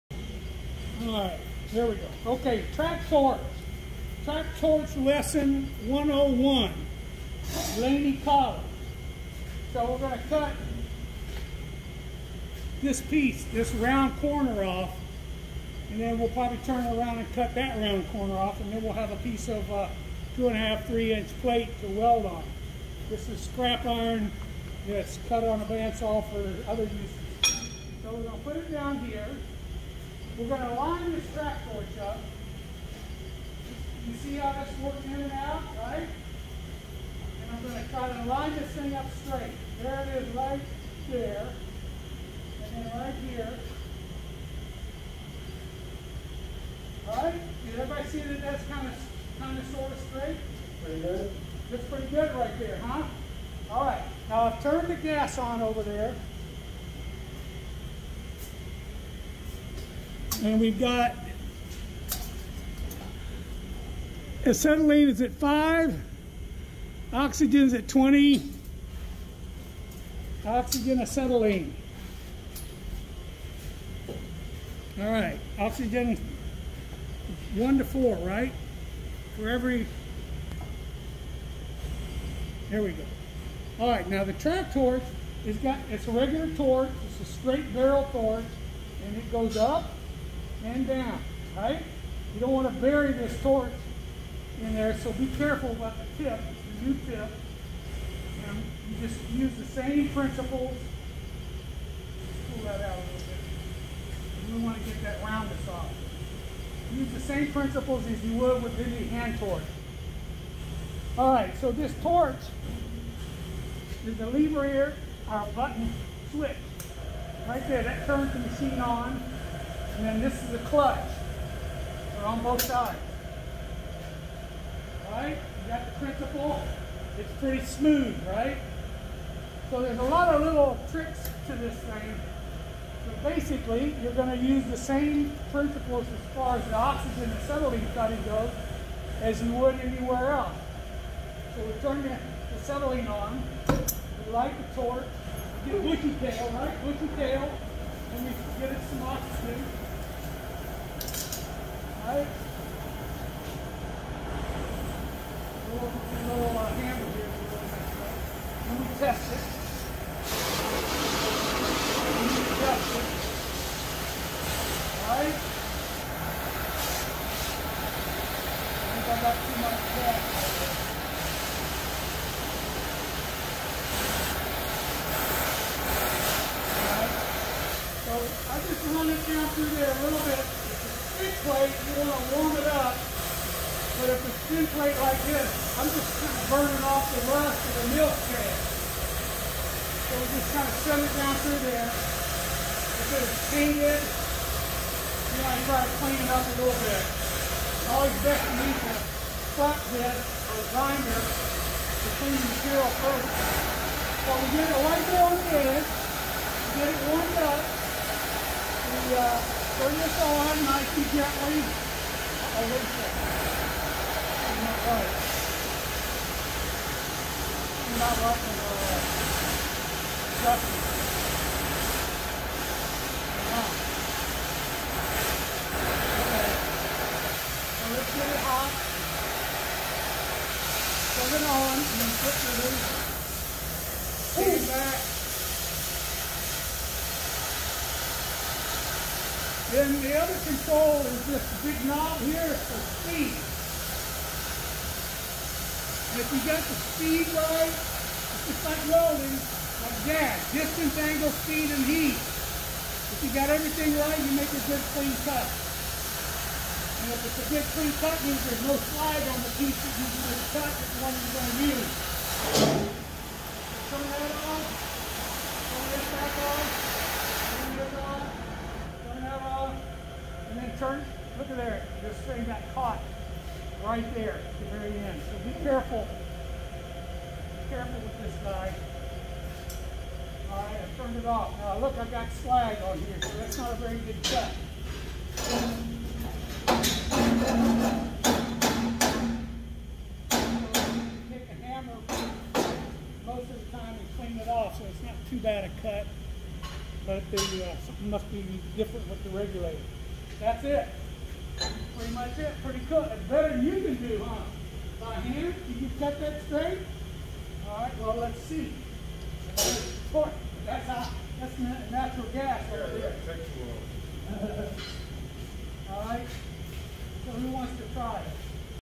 Simple Track Torch Lecture
Here's a quick lesson on operating a track torch.  It's pretty much the same as using a hand held oxy-acetlyene torch (aka, gas axe), except the straight barrel torch rides atop a motorized platform mounted on a track...just like the choo choo train you got for Christmas.
I hope this helps introduce you to a very valuable tool that all weldors should be super expert operators...just like with the hand held gas axe.
If you make sloppy cuts, your boss is not gonna like it, so practice, practice, practice until you get good at it.
And, Never Stop Learning!